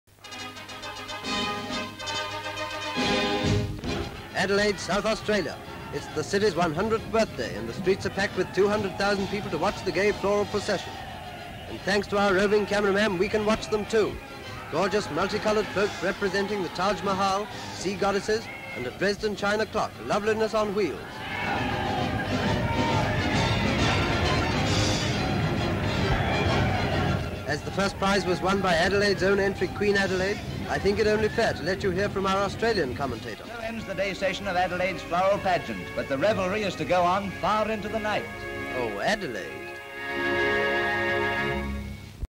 Adelaide celebrates 100 years of existence (1936)
Adelaide hosts floral pageant among other festivities during centenary celebration

AUSTRALIA: South Australia: Adelaide:
EXT

ADELAIDE Carnival on 100th Birthday - Florial Floats

CARNIVALS Adelaid's 100th birthday - Florial Floats

Celebrations - Anniversaries (Impersonal), Australia

festivals, pageant, parade, parades, celebration

Background: Adelaide hosts floral pageant among other festivities during centenary celebration